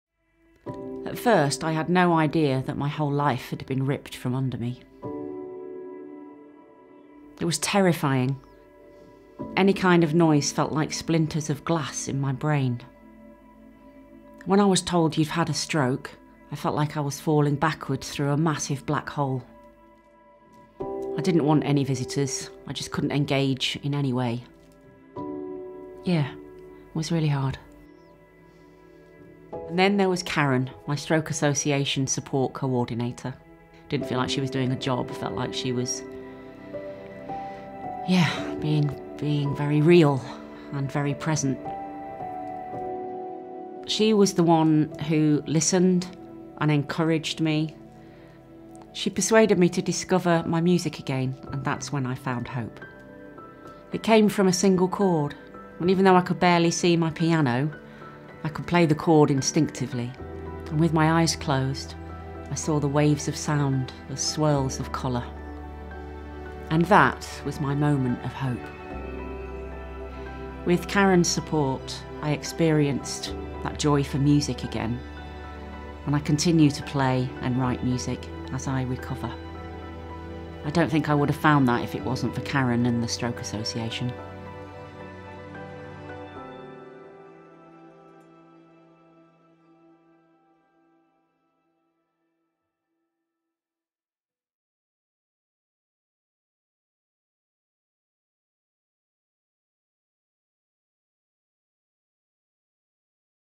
Hope After Stroke: Sarah Jay
Sarah Jay is a professional singer, songwriter and music teacher. After her stroke in 2015 she was told the devastating news that she’d never read again.
Sarah Jay says, “I thought my life was over. I know first-hand how critical hope is when rebuilding your life.” Her Stroke Association Support Coordinator, Karen, helped Sarah Jay find hope again throughout her stroke recovery.
Currently, not everyone gets the support and treatment they need to find hope and rebuild their lives. But by supporting the Stroke Association, you can help to change that.

Stroke Association. Rebuilding lives after stroke.